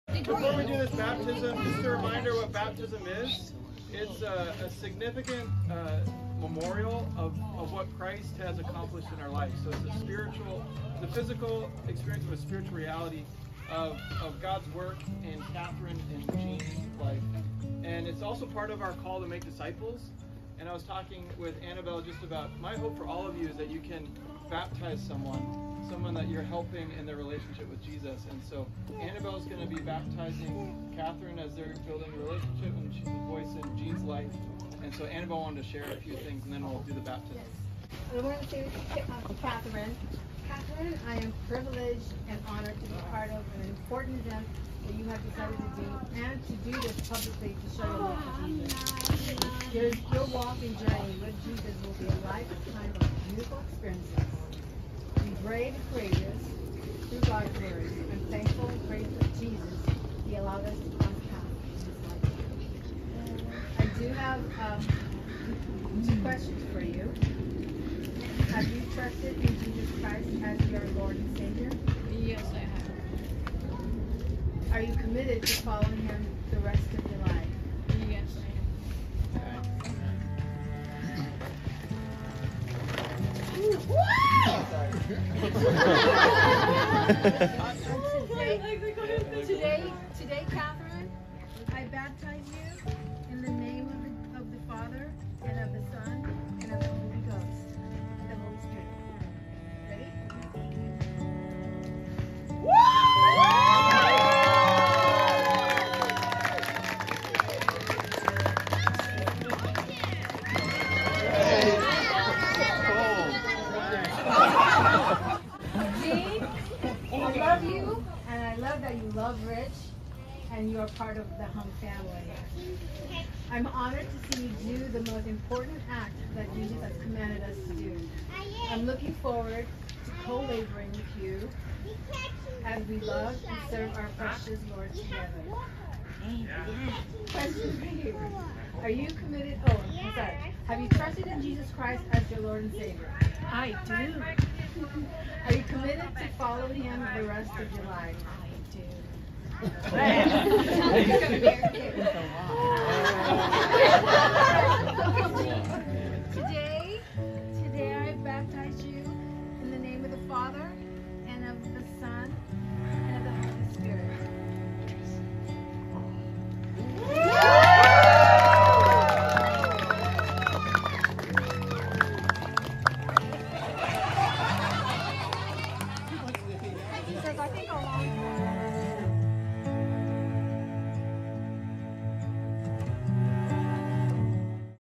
November 13, 2022 | Baptisms
Today we celebrated the new life in Christ for Katherine & Jean, who proclaimed their commitment to Christ through baptism on Sunday. Thanks to the many friends and family, who shared in this special time.